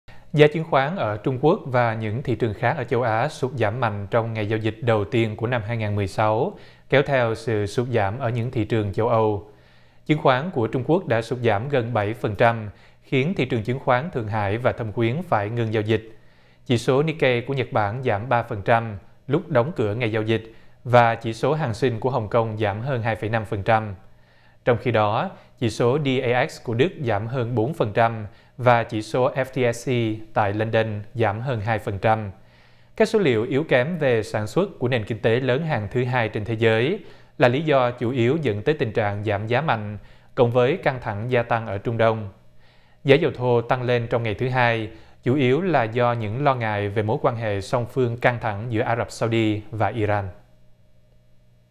Chứng khoán thế giới sụt giảm vì lo ngại về kinh tế Trung Quốc
Tin tức Nếu không vào được VOA, xin các bạn hãy vào để vượt tường lửa. Giá chứng khoán ở Trung Quốc và những thị trường khác ở châu Á sụt giảm mạnh trong ngày giao dịch đầu tiên của năm 2016, kéo theo sự sụt giảm ở những thị trường châu Âu.

Chứng khoán của Trung Quốc đã sụt giảm gần 7% khiến thị trường chứng khoán Thượng Hải và Thâm Quyến phải ngưng giao dịch.

Chỉ số Nikkei của Nhật Bản giảm 3% lúc đóng cửa ngày giao dịch, và chỉ số Hằng Sinh của Hong Kong giảm hơn 2,5%.

Trong khi đó, chỉ số DAX của Đức giảm hơn 4%, và chỉ số FTSE tại London giảm hơn 2%.

Các số liệu yếu kém về sản xuất của nền kinh tế lớn hàng thứ hai trên thế giới là lý do chủ yếu dẫn tới tình trạng giảm giá mạnh, cộng với căng thẳng gia tăng ở Trung Đông.

Giá dầu thô tăng lên trong ngày thứ Hai, chủ yếu là do những lo ngại về mối quan hệ song phương căng thẳng giữa Ả-rập Saudi và Iran.